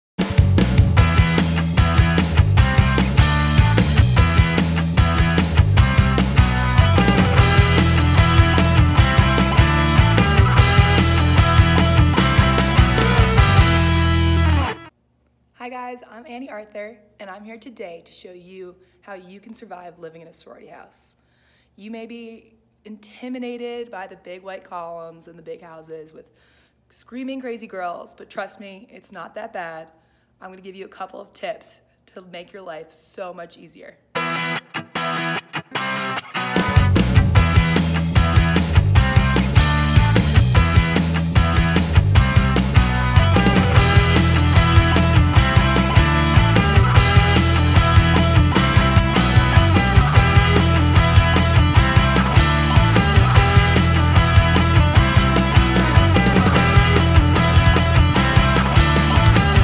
What is it really like to live in a sorority house?
Are you scared of the thought of living in a house with thirty of your best friends? Are you nervous you won't be able to survive? Look no further this video will give you the steps on how to live in a sorority house.